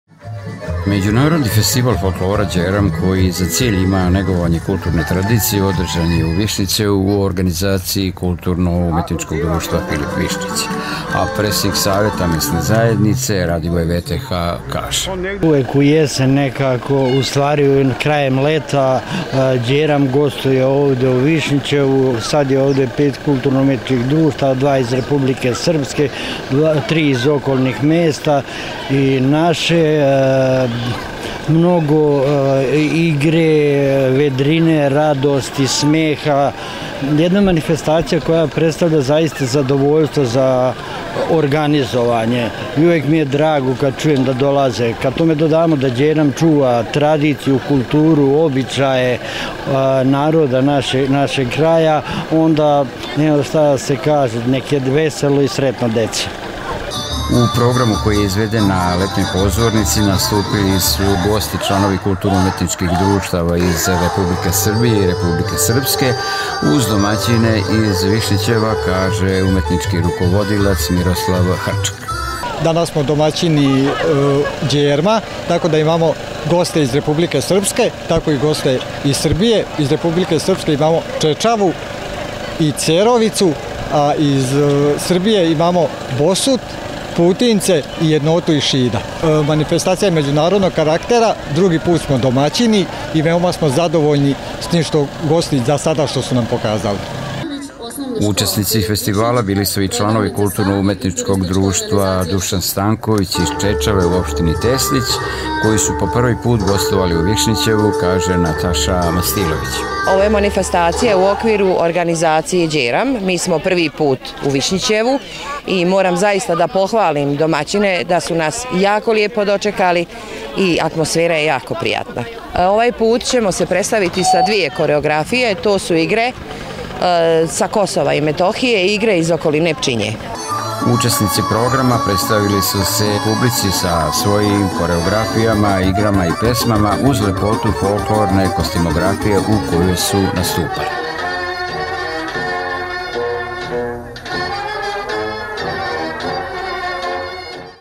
Đeram u Višnjićevu
Kulturno umetničko društvo „Filip Višnjić” iz Višnjićeva bilo je domaćin Međunarodnog festivala folklora „Đeram”, koji za cilj ima negovanje kulturne tradicije. U programu koji je izveden na letnjoj pozornici nastupili su gosti iz kulturno umetničkih društava iz Bosuta, Boljanića, Čečave, Cerovice i Jednote iz Šida.